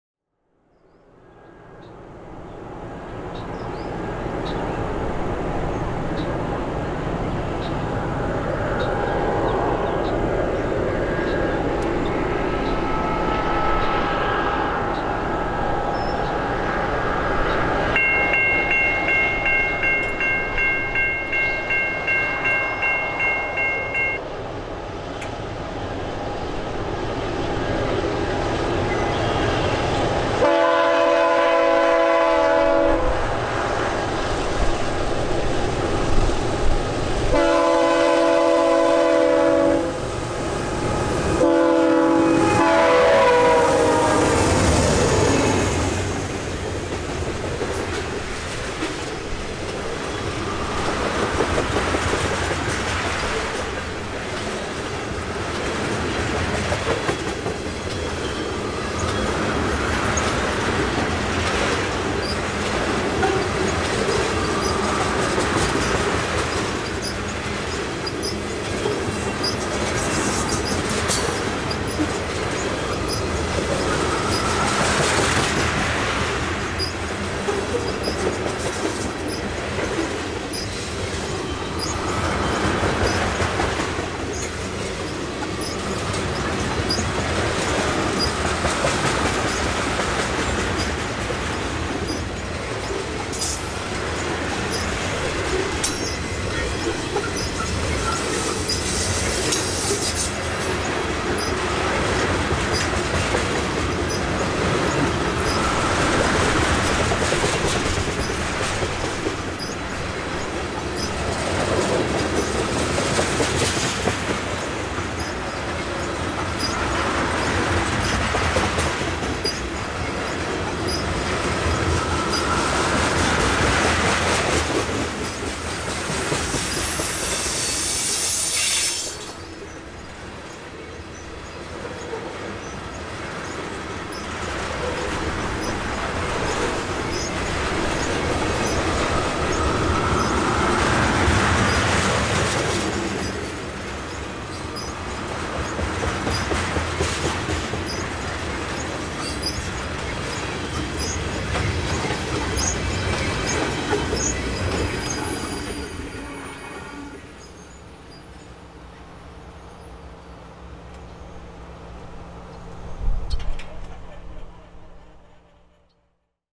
GECX 2043 East - Aerial Drone Views on 5-26-2018
A blue GE ES44AC is in the lead of an ethanol train on the Canadian National's Freeport subdivision.